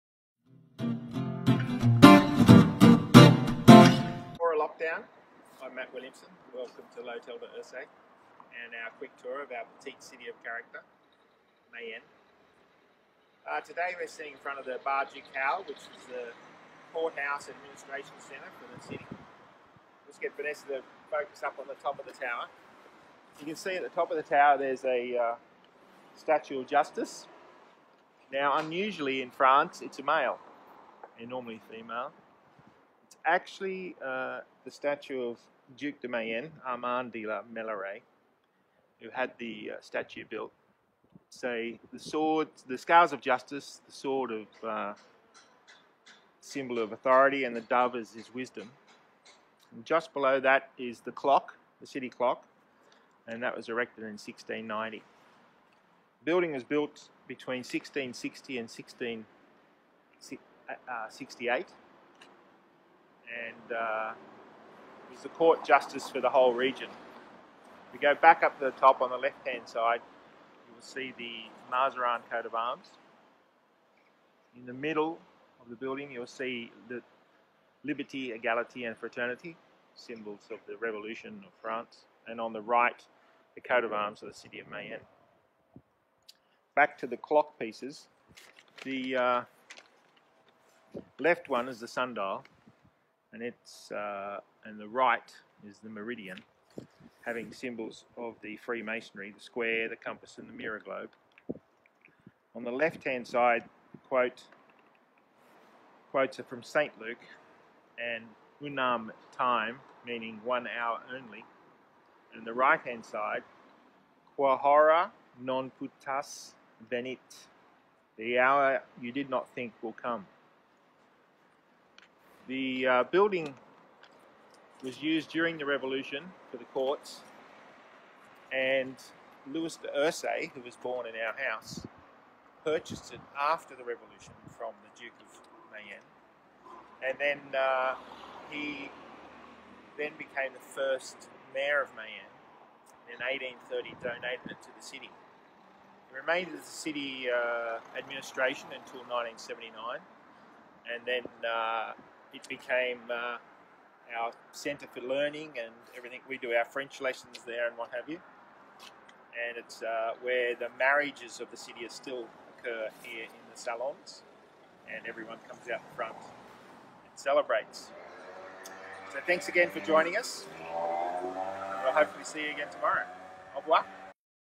Day 4. Barre Ducal Palace, Mayenne Virtual Tour
Lockdown Tours of the city of Mayenne Pays de Loire France
Today we visit the ancient Mairie or Town Hall built at the end of the 17thc.
By Vanessa and Matt Williamson of L'Hotel de Hercé Mayenne